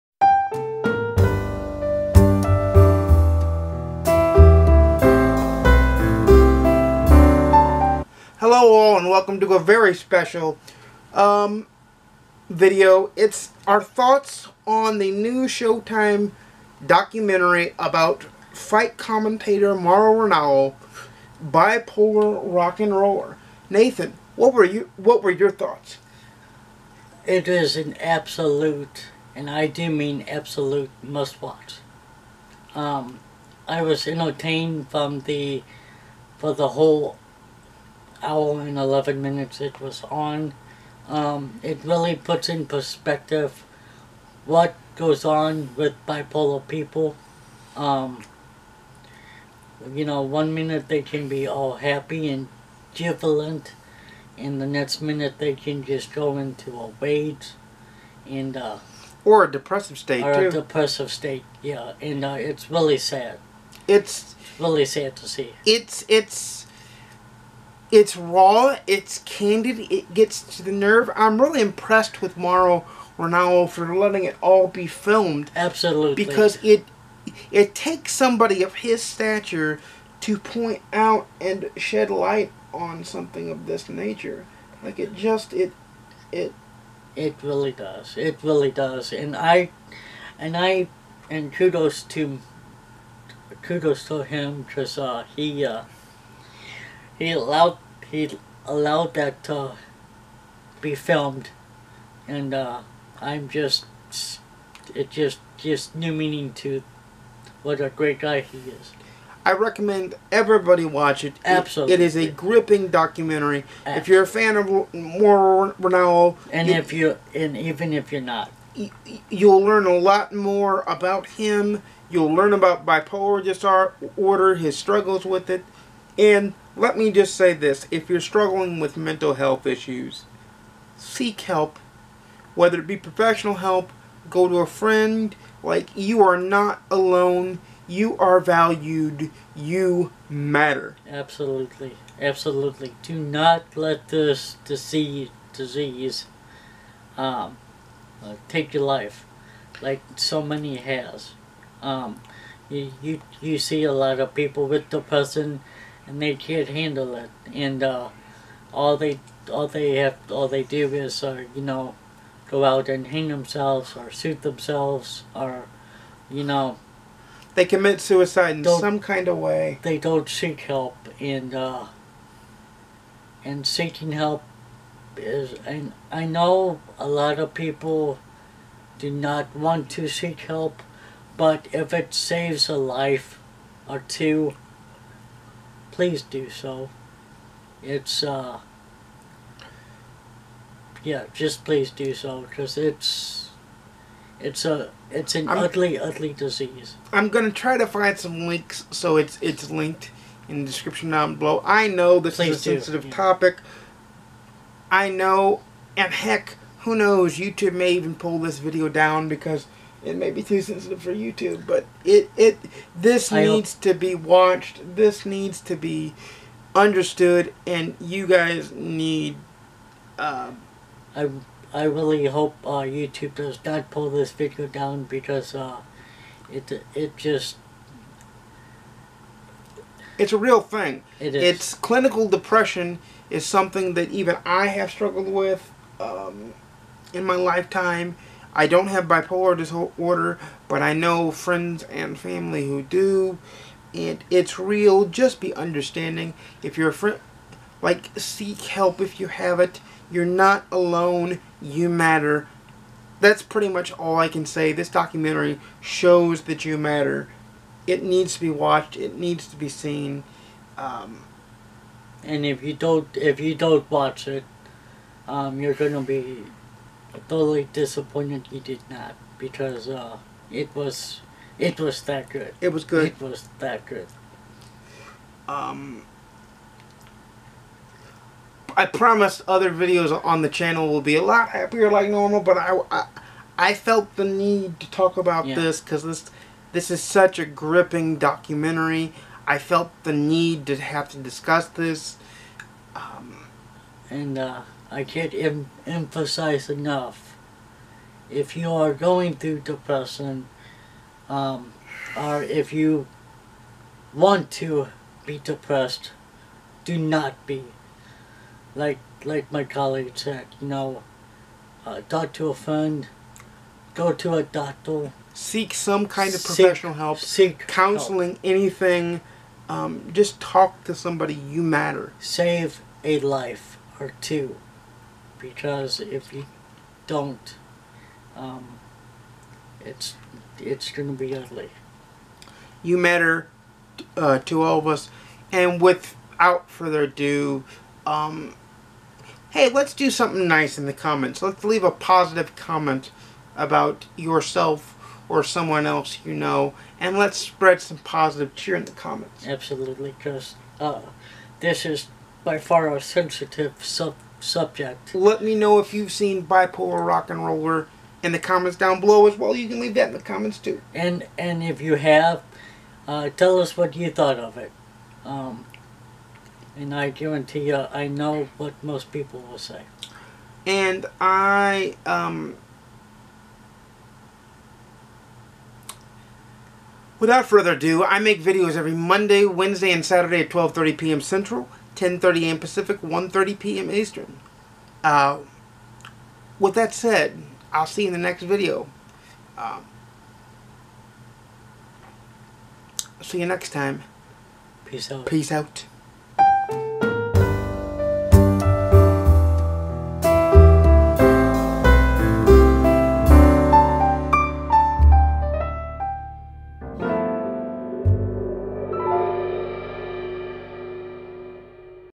Bipolar Rock n Roller Documentary Discussion
Nate the Wrestling Dude and I Dive into the discussion of the Showtime documentary Biopolar Rock 'n' Roller.  Mauro Ranallo the subject of the film is a famous sports caster with the titular disorder. If you are suffer from mental health issues you are not alone.

A link if you need help: You are not alone